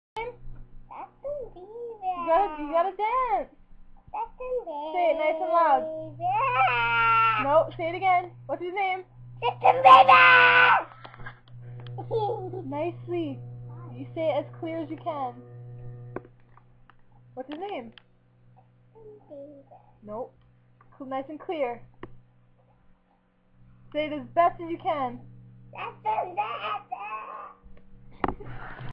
DUSTIN BEAVER
Apparently my 3 yr old cousin knows who Justin Bieber is, ahah(: well, sorta...